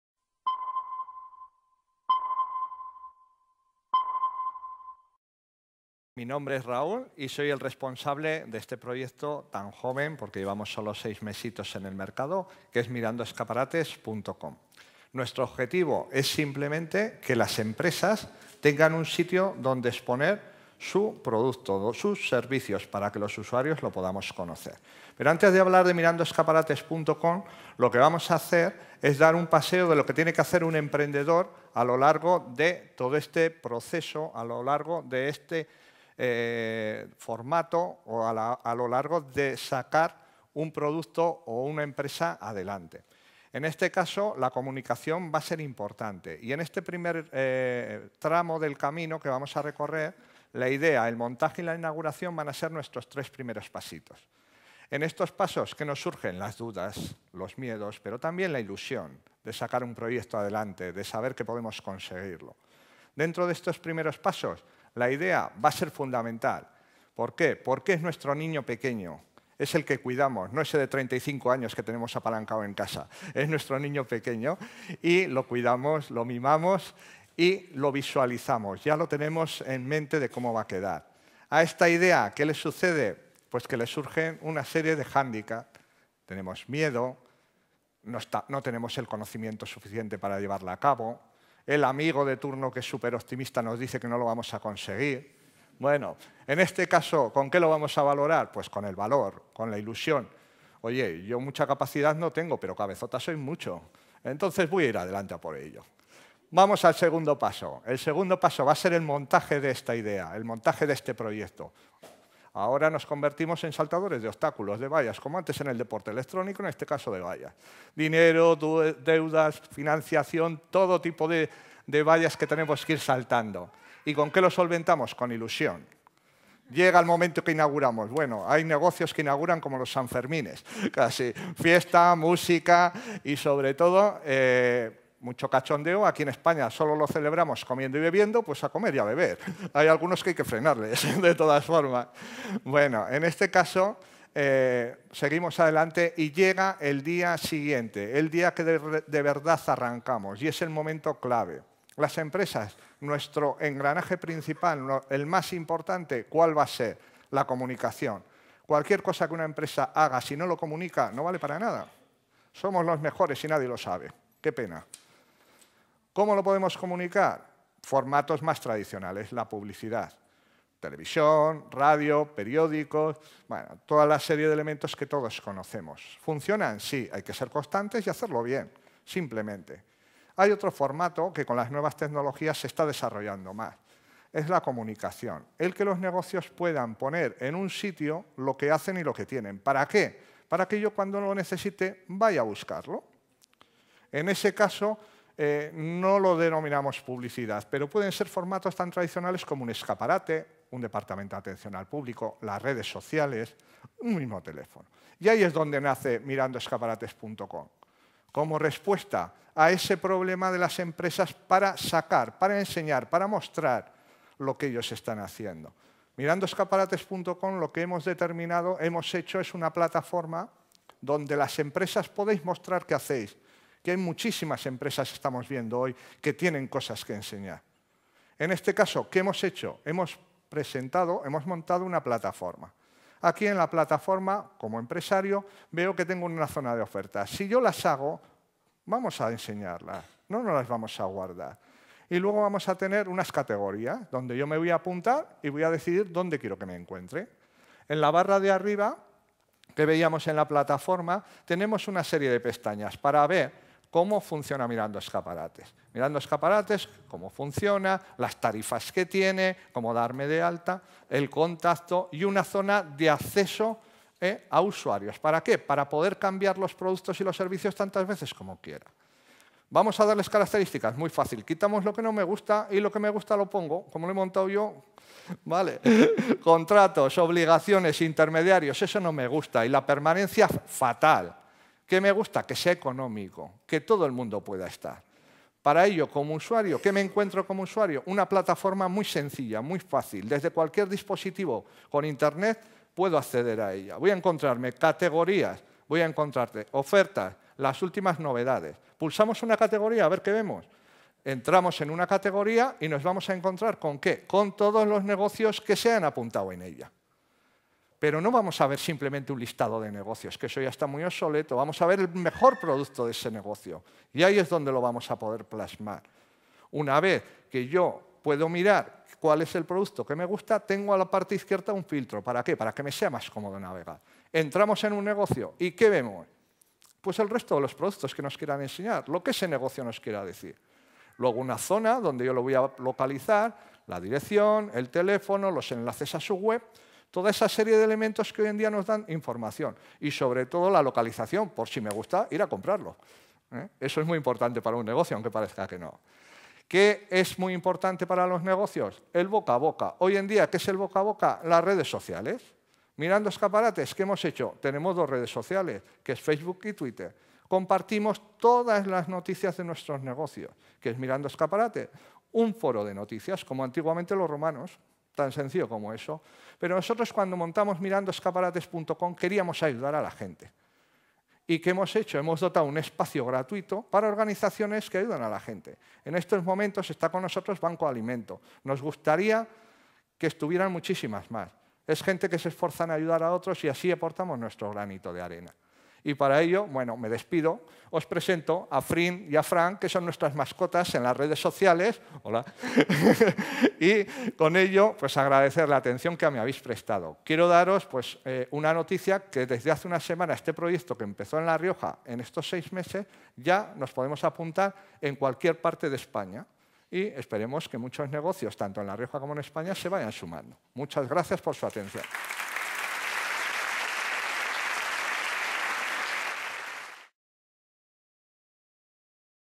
Raúl González | PechaKucha Night Logroño
14ª Edición PechaKucha Night en Logroño, 10 abril 2014.
Showroom Pancorbo. Galería Arteaga.

Fundación Riojana para la Innovación